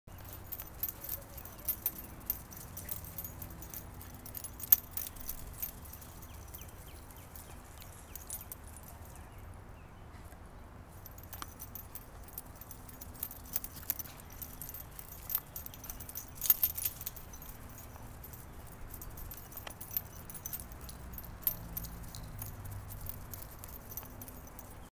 Dingo herding the dogs
Dingo (ACD) herds the rest of the crew in the yard